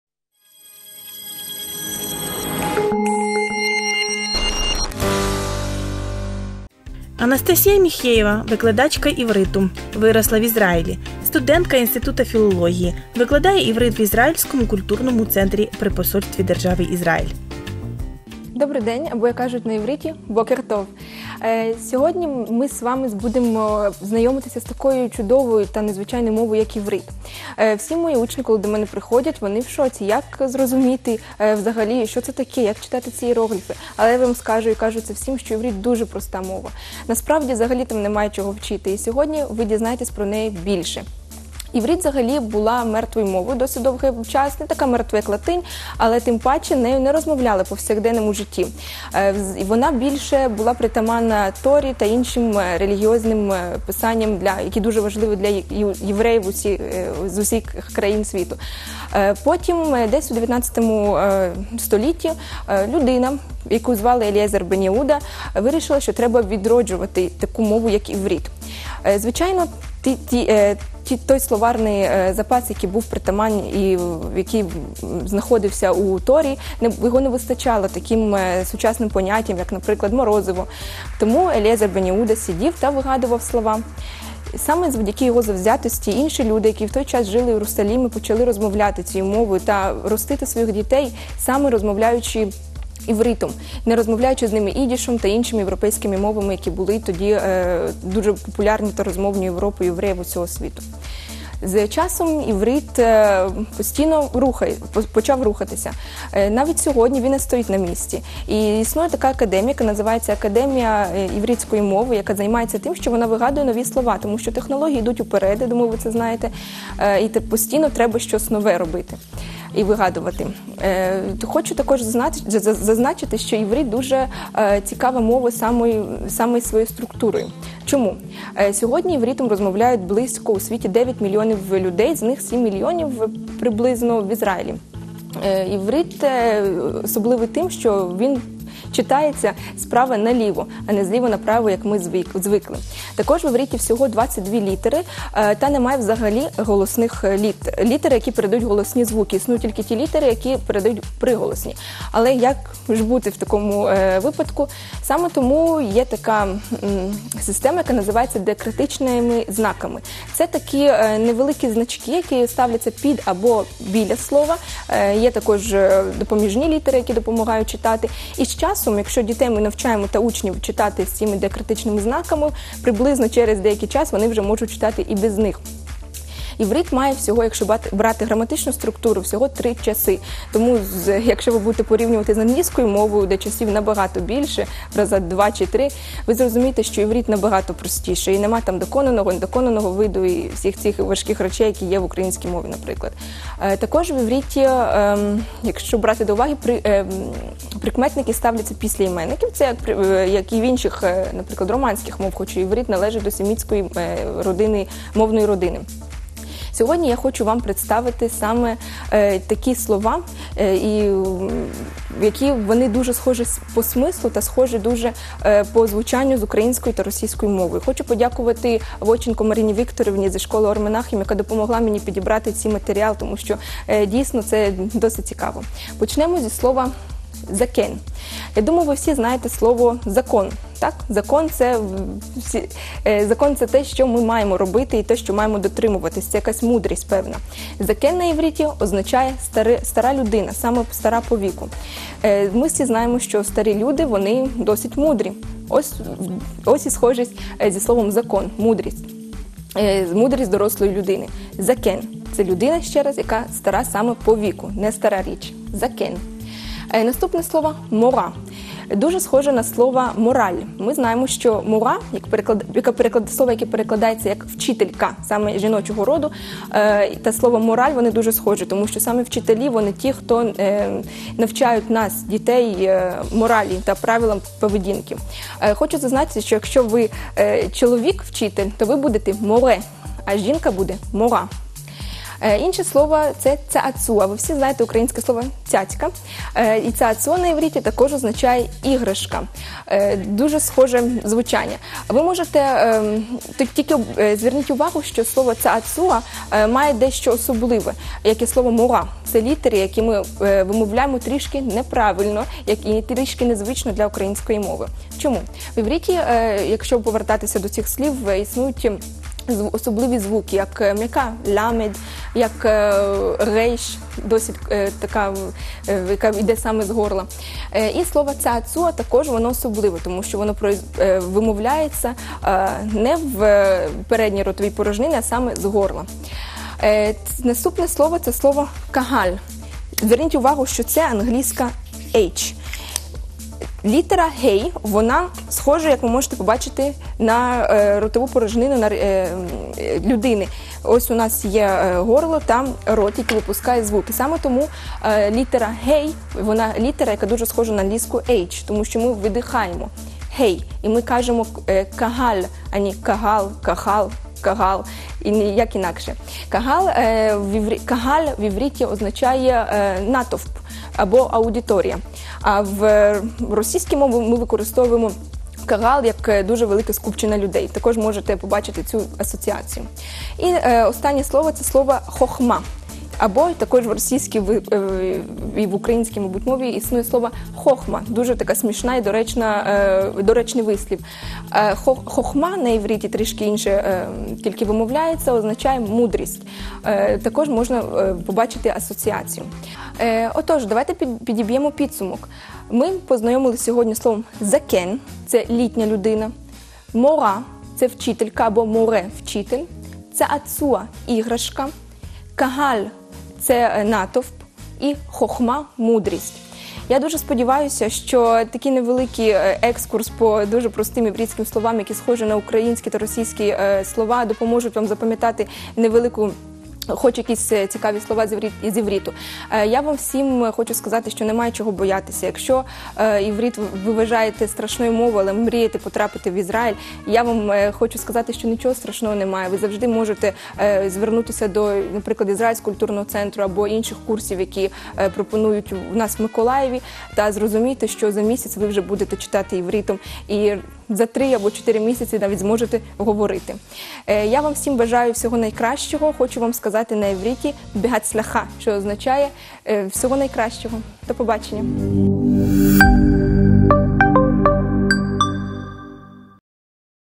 05.11.18. Новий день. Рубрика "Слово дня": іврит
П’ять слів з івриту, співзвучних із українськими, але інакші за значенням.
Вчимо іврит разом із викладачкою Анастасією Міхєєвою.
Що означають слова מורה (МОРА), זקן (ЗАКЕН), צעצוע (ЦААЦУА), קהל (КАНАЛ), חוכמה (ХОХМА)?